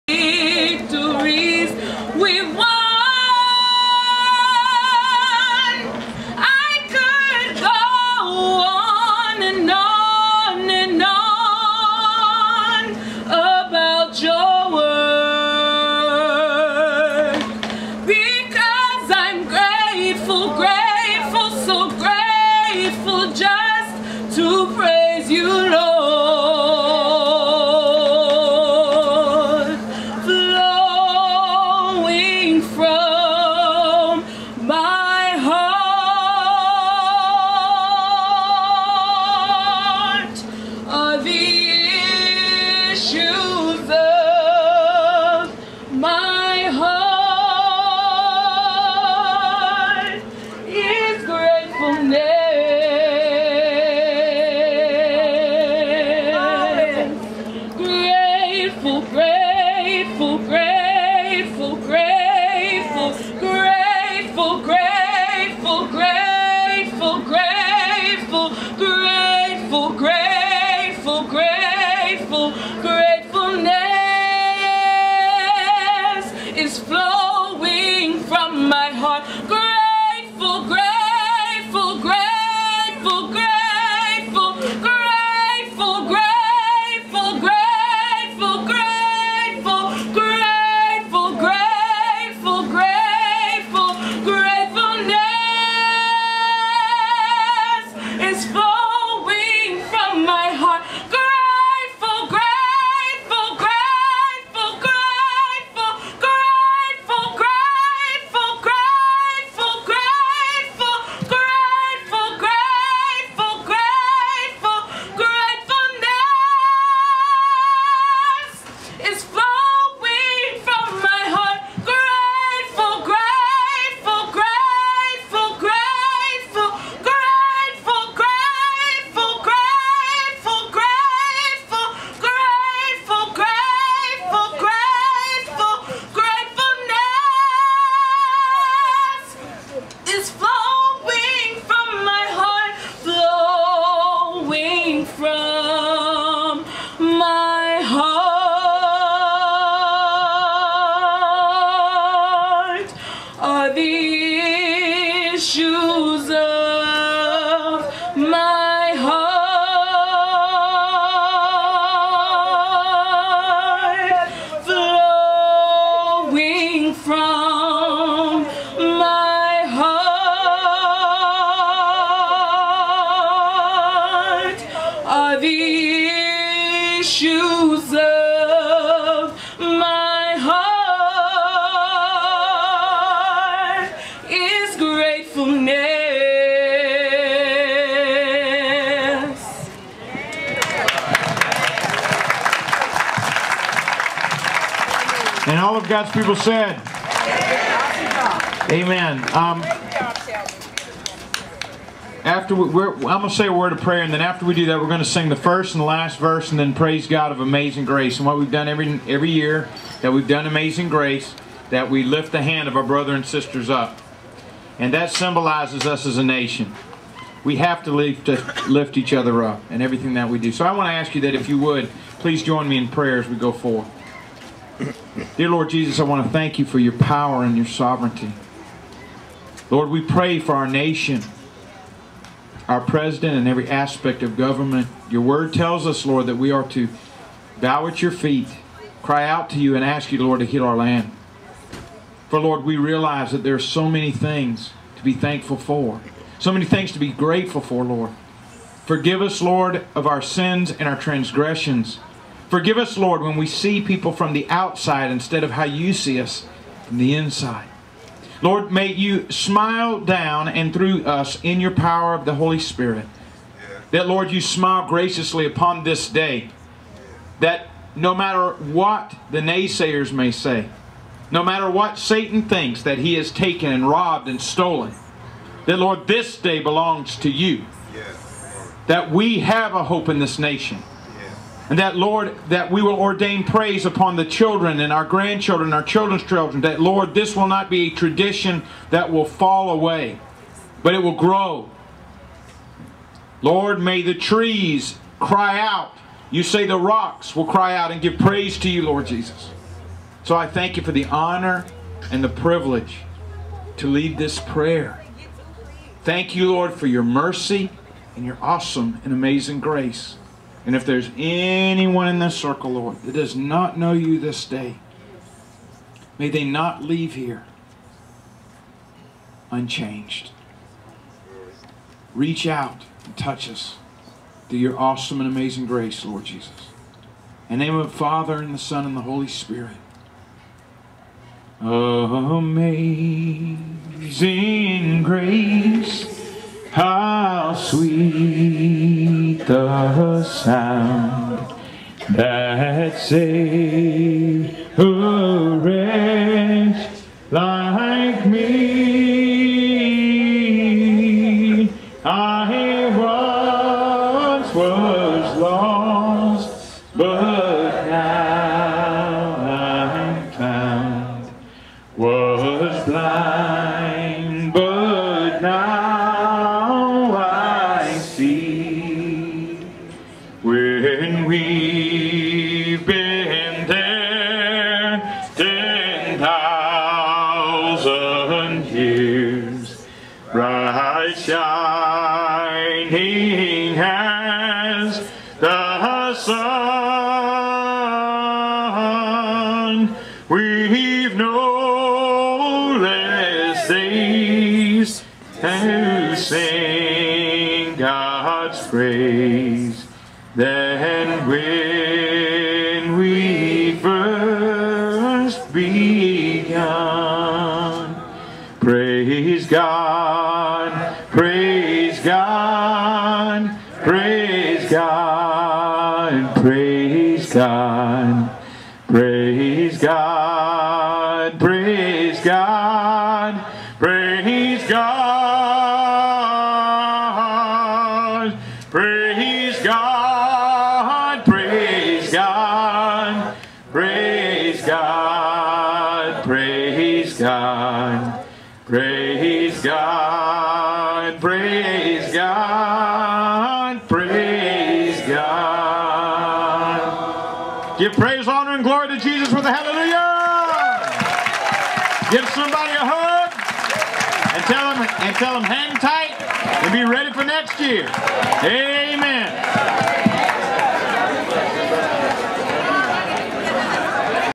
FTM - 13 Z
Finale - Feeding of the Multitude event, Johns Island, South Carolina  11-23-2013.